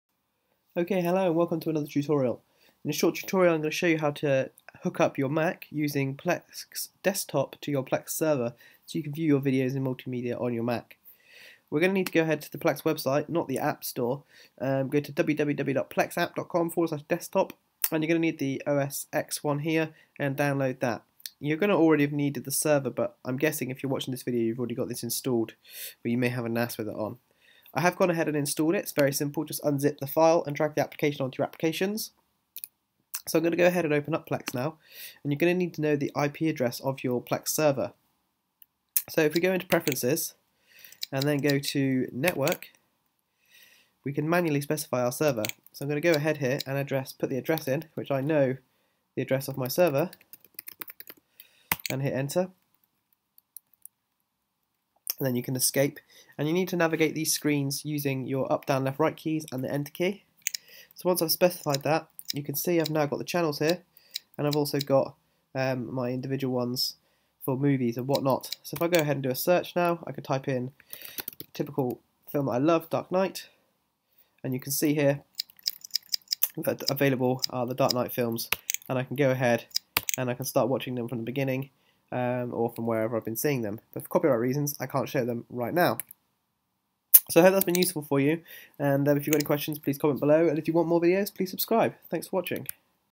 How To Connect Plex Desktop on Mac OS X to Plex Server [HD] 2023
This short tutorial will show you how to setup Plex Desktop on your Mac OS X machine if you already have a Plex Server configured on a separate machine or NAS elsewhere in your network. Hope this is useful.